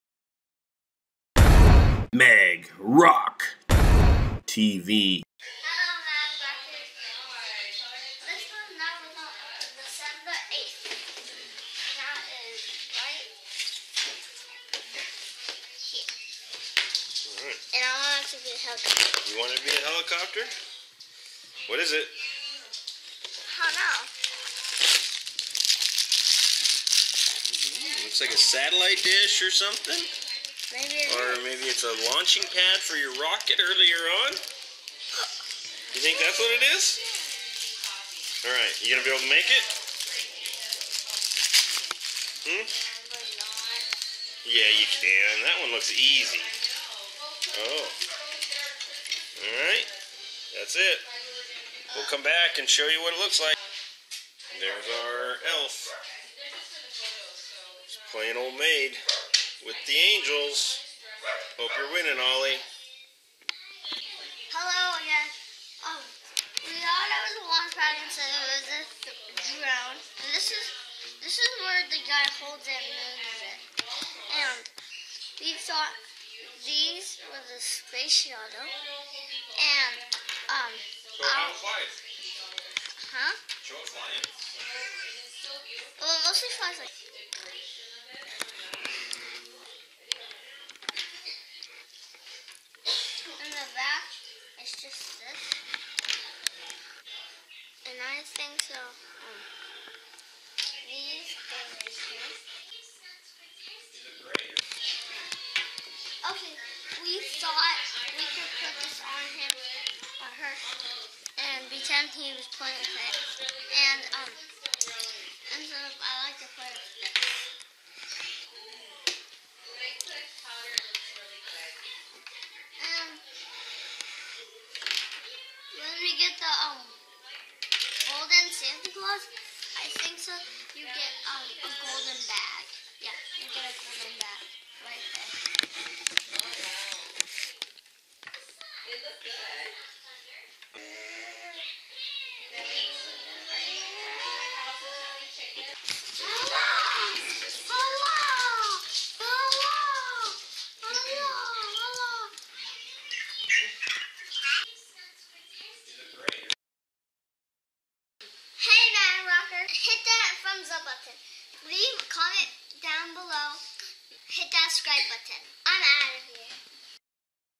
December 8th Lego City Advent
Roc opens day 8 - December 8th - of his Lego City Advent Calendar and gets a Drone.  Ali plays old maid with the angels.